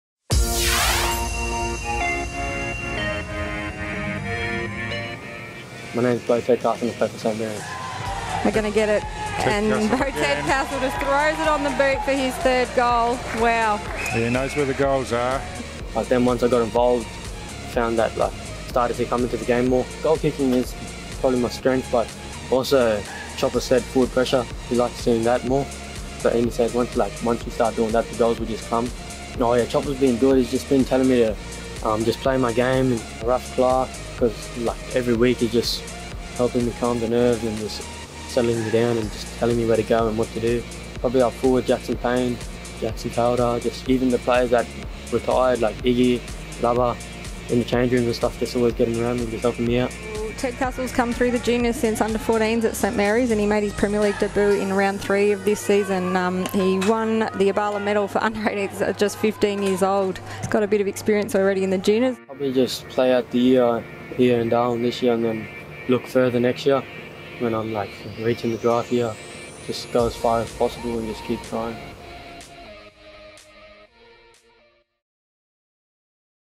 2020/21 NT News Rising Star - Round 5 Nominee: Beau Tedcastle
He's only 16-years old, but St Mary's young gun, Beau Tedcastle, has been turning heads with his goalkicking ability and defensive pressure. After a four-goal haul on the weekend, he's been nominated for The NT News Rising Star! Good on ya Beau! May not be reproduced by any means without the express permission of the rights holder.